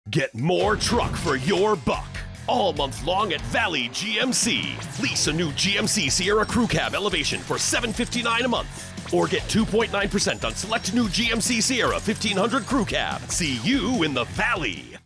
New 2024 GMC Sierra 1500 on Sale at Valley GMC
🌟 Discover New 2024 GMC Sierra – On Sale Now! 🌟

Get ready to elevate your driving experience with the all-new 2024 GMC Sierra, now available at incredible savings! Designed to deliver power, capability, and innovation, the 2024 Sierra redefines what it means to drive a pickup truck.

Equipped with advanced performance features, including powerful engine options, rugged towing capabilities, and a smooth, responsive ride. Whether you're tackling tough terrain or cruising through city streets, the GMC Sierra delivers unmatched performance and reliability.

See you in the Valley!!
VALLEY GMC IN AUBURN